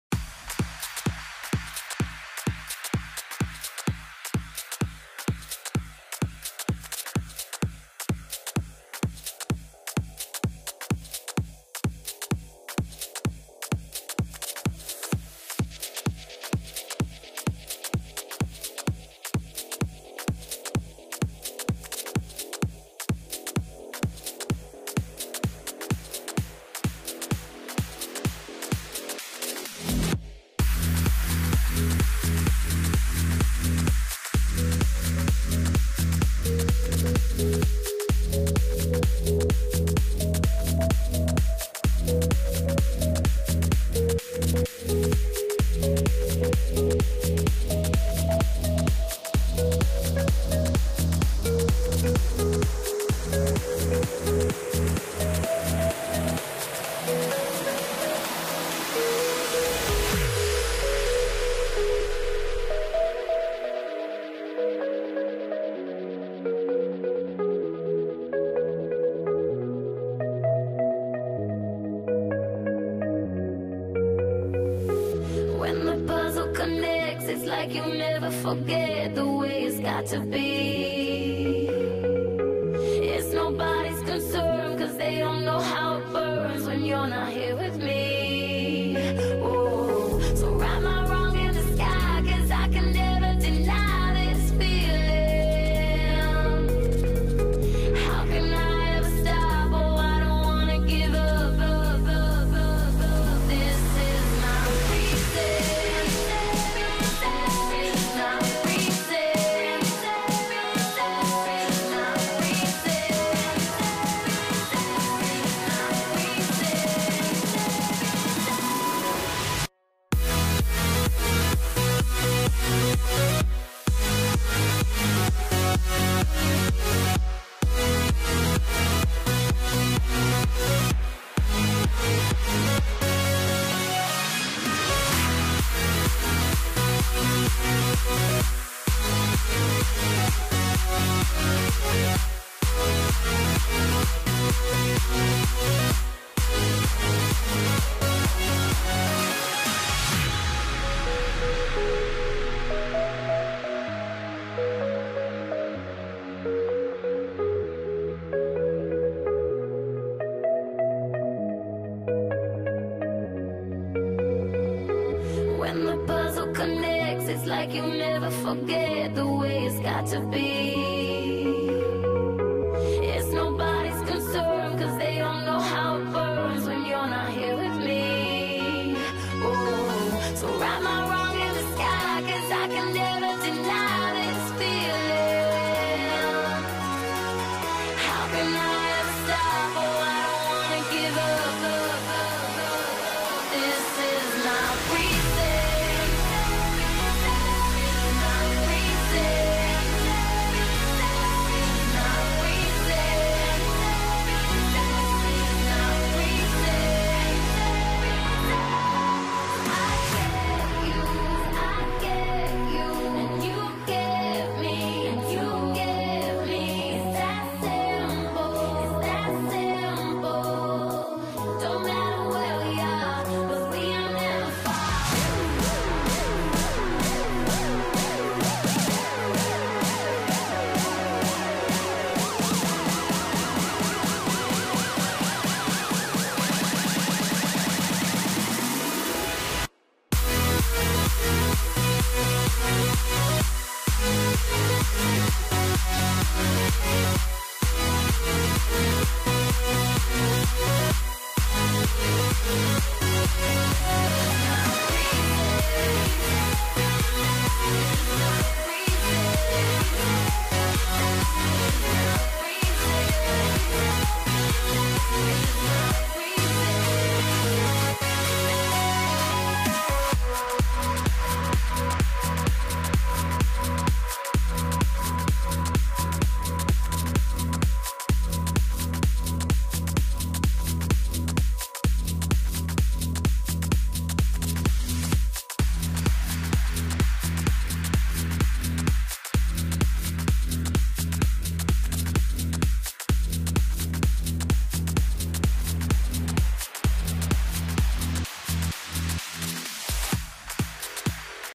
Xbox 360 Controller Speed Art in Photoshop
I create an Xbox 360 controller in Adobe Photoshop. Download link of the project file is included, enjoy.